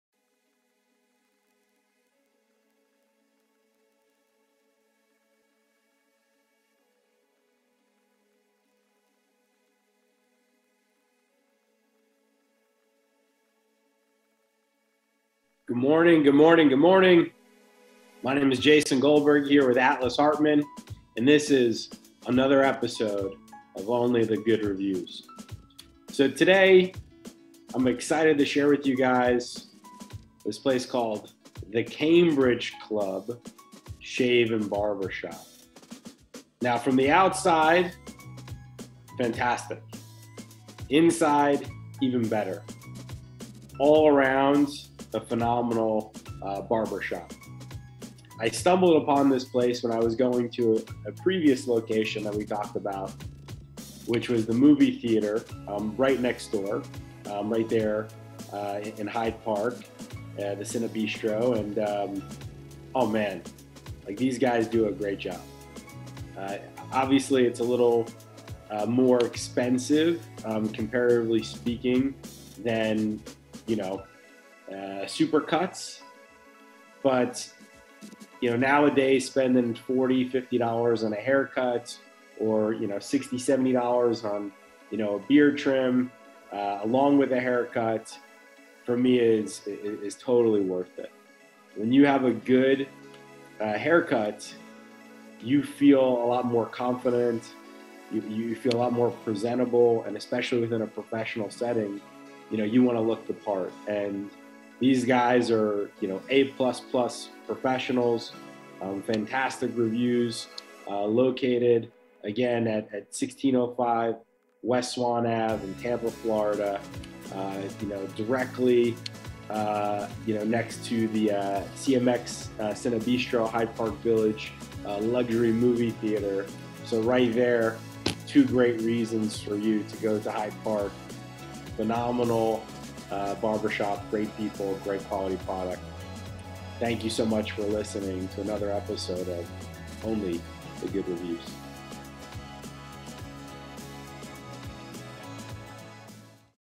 Only The Good Reviews - Tampa Part 14 | BARBERSHOP | The Cambridge Club Shave & Barbershop
Next up on the Only The Good Reviews (Tampa) agenda, we're sharing The Cambridge Club Shave & Barbershop! 💇 ‍♂️ With its sleek and modern look, it impresses right away. What stands out to Atlas Hartmann about this small business is their professionalism and expertise. When you have a good haircut, you feel confident, and we recommend this place to try!